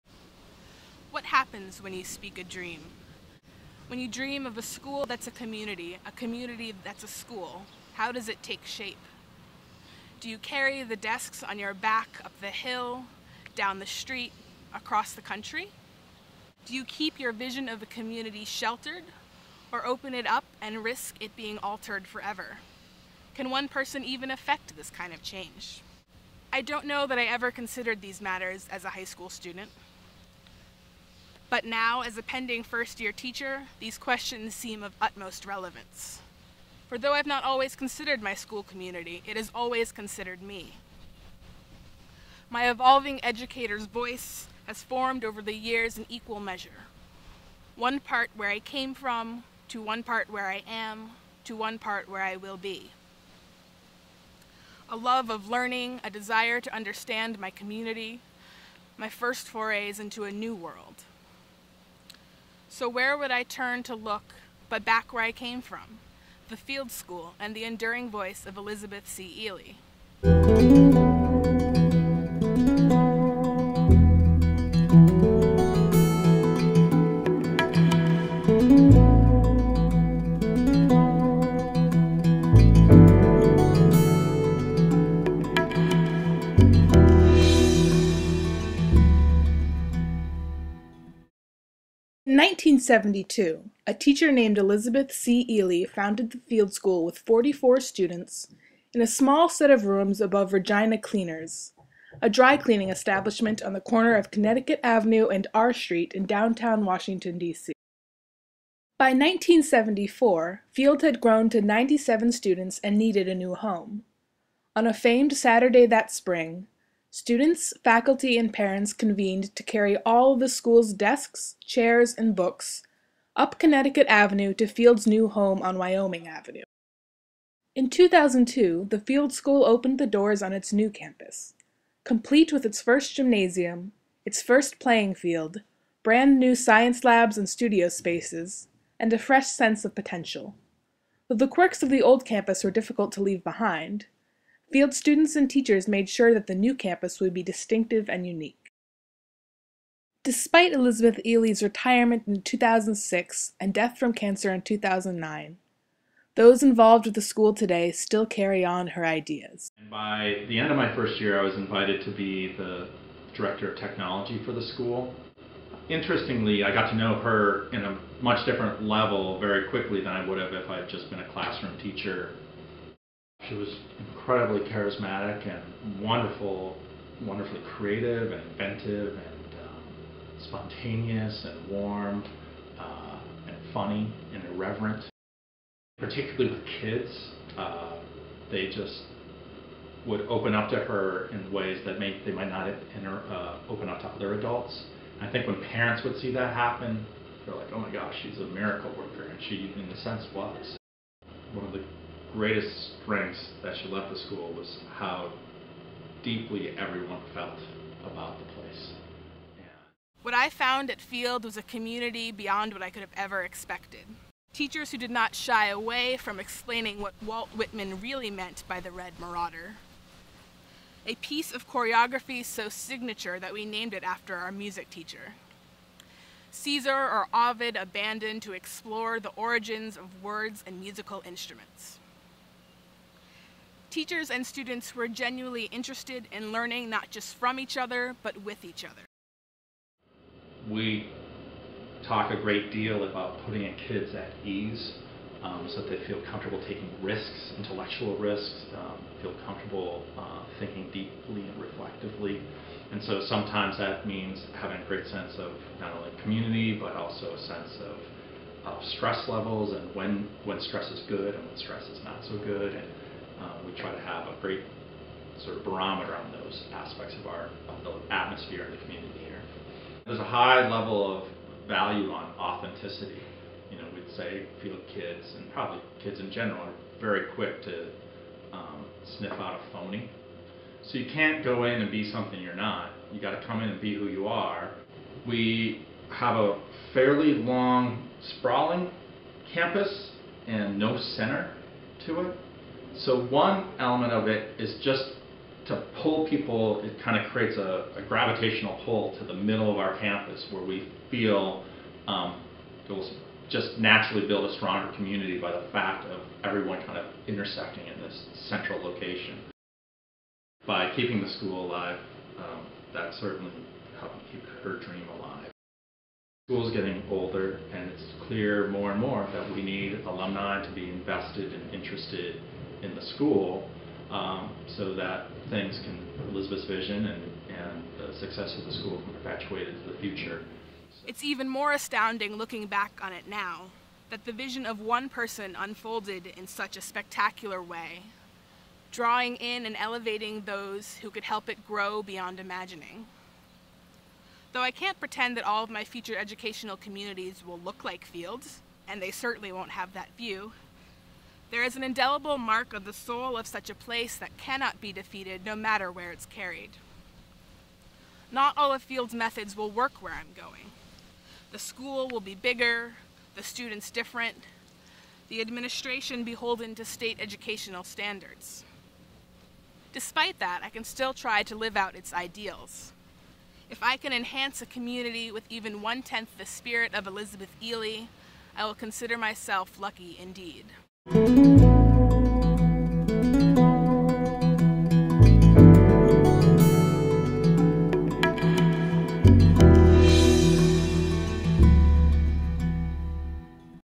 Because She Dared
To leverage the art of community video storytelling as an innovative tool for developing professional identity and community, enhancing adolescent growth and learning, and meeting core requirements and standards, the George Washington University's Graduate School of Education and Human Development (GSEHD) and Meridian Hill Pictures in Washington, DC, piloted Community-Engaged Teacher as Artist over the Summer 2013. The project integrates advanced participatory documentary film techniques into the training of core content area teachers in GSEHD's Secondary Education M.Ed. Community-Engaged Teaching (CET) program. The pilot project supports and fosters the development of new teachers during a capstone course by training teachers in how to use media arts and documentary storytelling to support the creation of a community engaged learning environment for their students.